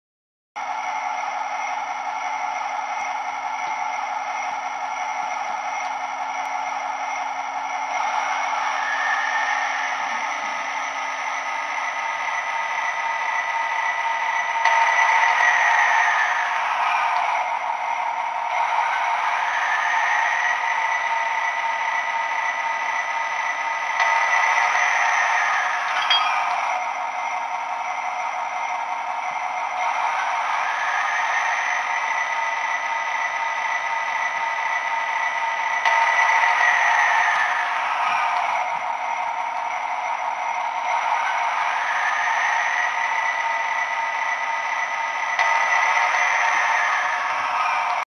Oświetlenie manewrowe w SU45 PIKO z dekoderem TS.f091-i256 za 150zł
Dekoder oferuje także wygaszanie czerwonych, światła długie, krótkie, postojowe i alarmowe.
Dekoder dostępny z dźwiękami Taurusa, parowozów, EZT. Baza dźwięków jest stale powiększana. Więcej o dekoderze na